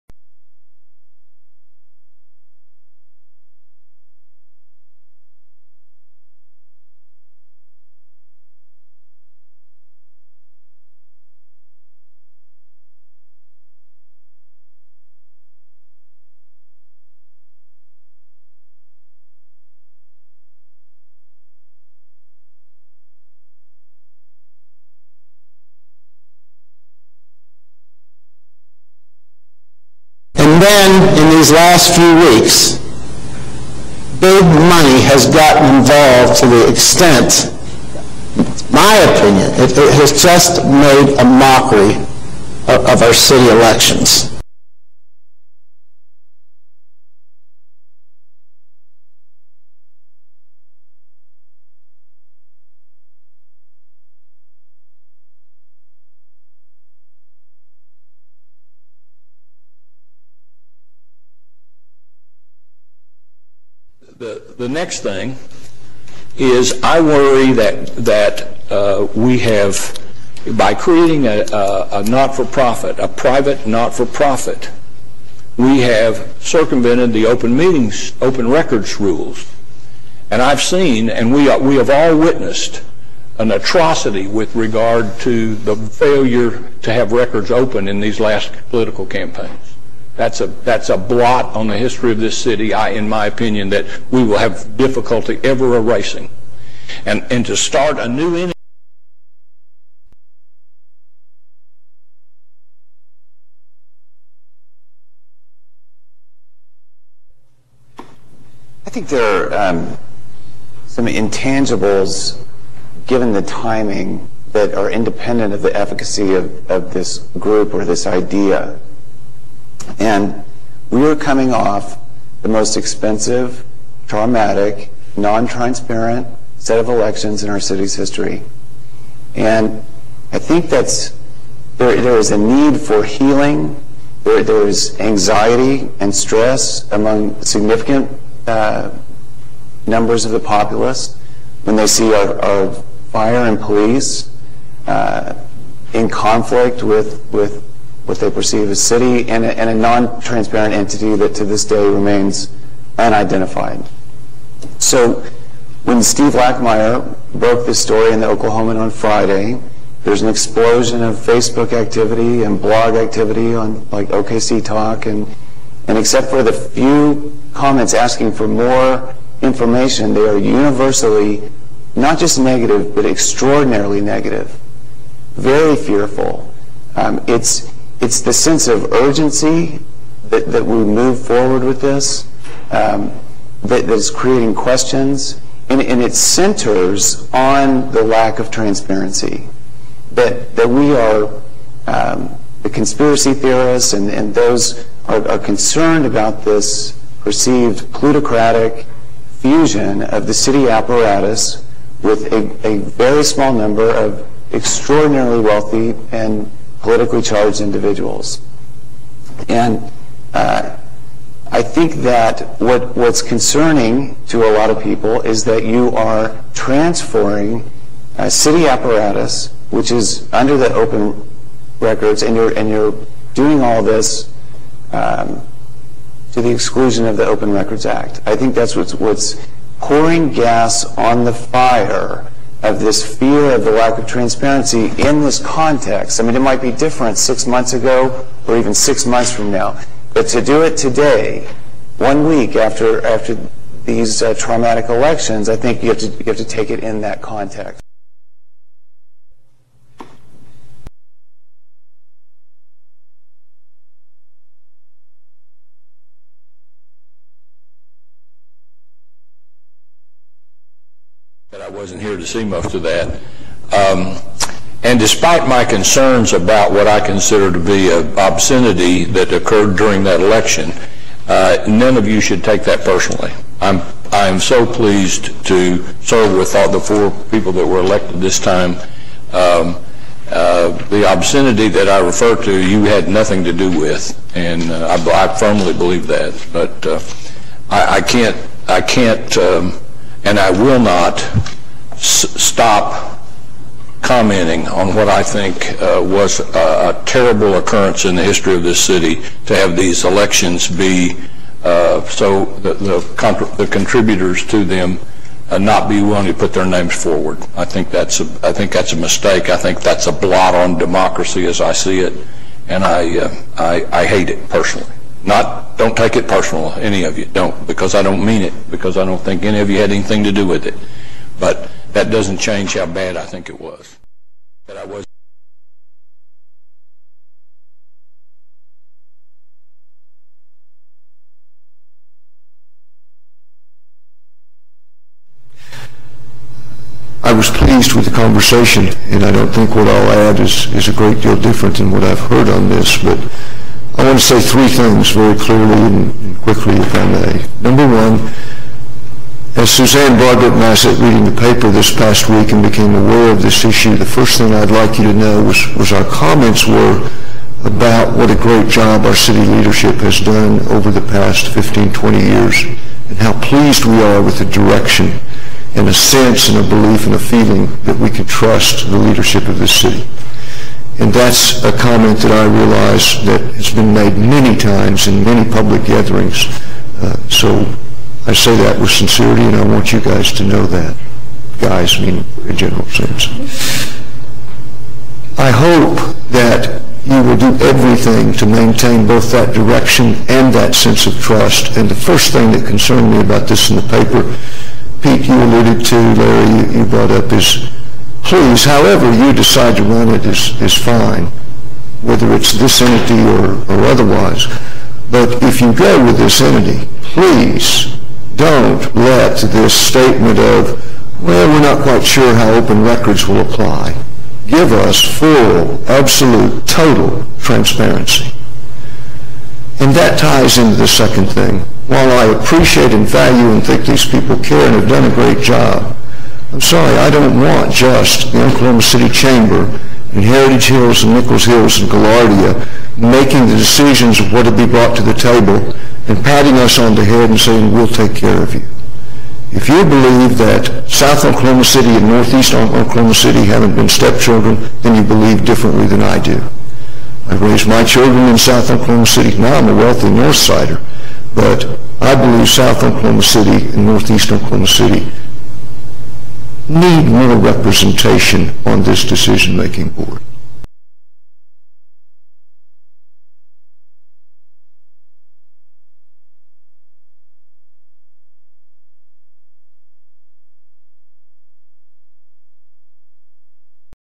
Re the Proposed Alliance & the Momentum Committee
This is a collection of remarks made at the 4/12/2011 City Council meeting as relates to the discussion associated with the proposed Alliance and the impact of the Committee for Oklahoma City Momentum on that consideration. Remarks are by Sam Bowman (3/1) and Pete White, Ed Shadid, & Dr. Jack Warner on 4/12/2011.